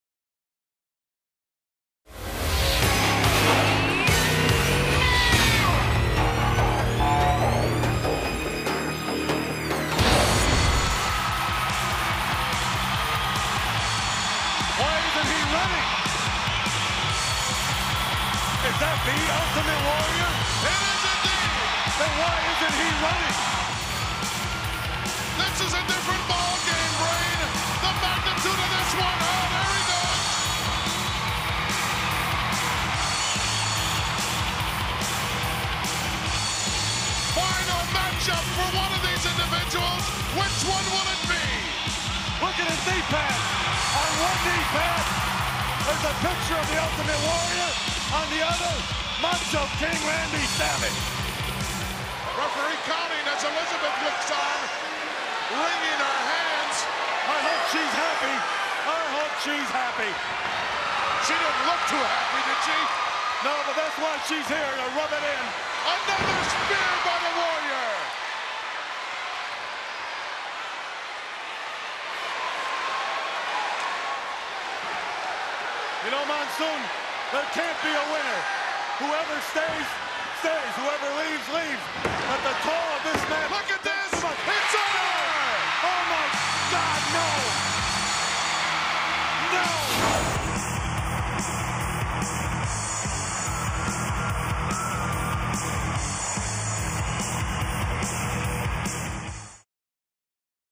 WWE Mattel Defining Moments: The Ultimate Warrior
WWE Mattel Defining Moments: The Ultimate Warrior competes against Randy Savage at WrestleMania VII